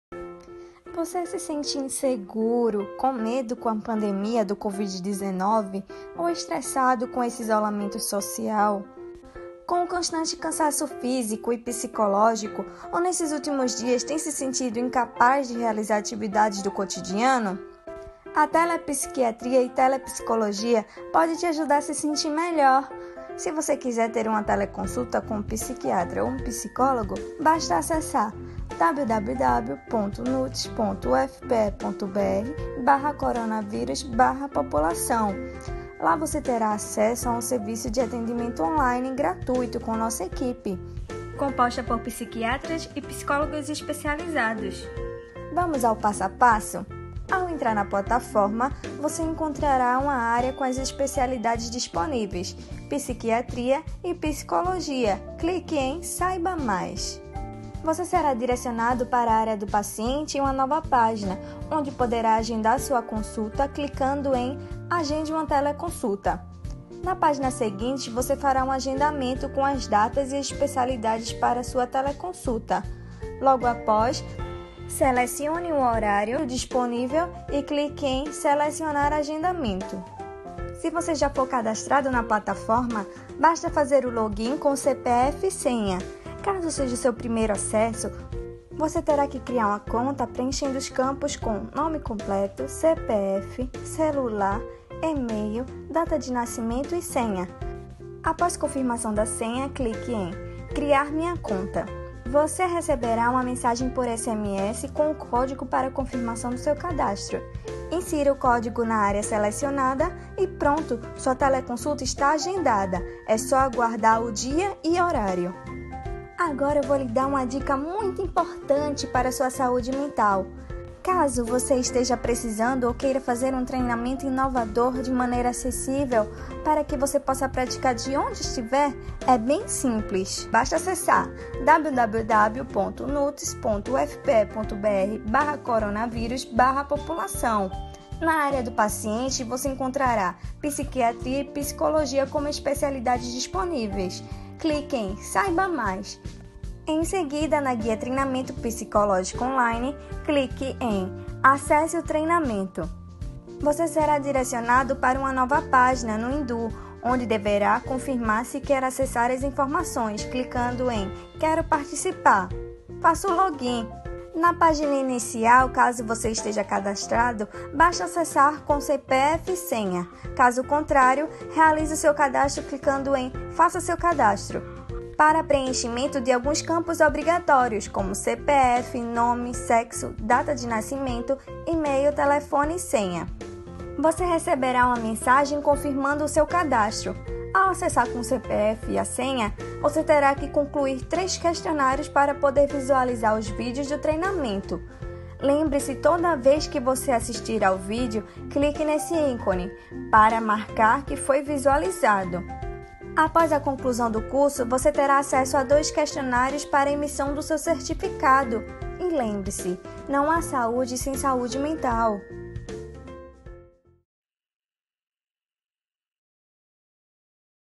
CTC - Telepsiquiatria e Telepsicologia (Nutes/UFPE)
Esse vídeo descreve o serviço de Telepsiquiatria e Telepsicologia da Central de Telemonitoramento Clínico desenvolvido pelo Núcleo de Telessaúde da Universidade Federal de Pernambuco (Nutes/UFPE)